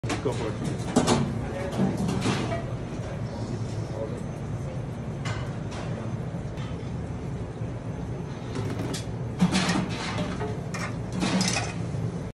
What’s your favorite way to eat Octopus? 🐙
For a light cooking method, try boiling your octopus in a large stockpot. Fill the pot 2/3 full with either a vegetable/seafood stock or water filled with the herbs and vegetables of your choice, helping to infuse flavor into the meat.

Once the liquid has been at a rolling boil with the vegetables and herbs for about 5 minutes, add your pre-cut octopus to the pot and cover. Gently boil the octopus for about 15-20 minutes per pound of octopus, testing the texture with a fork every 10-15 minutes until it has become fully tender and ready to serve. This method can additionally be used as a simple first step to tenderize your octopus before finishing it in a pan or on the grill for additional flavor and texture.

📍: The Sicilian Butcher
🌇: Phoenix, AZ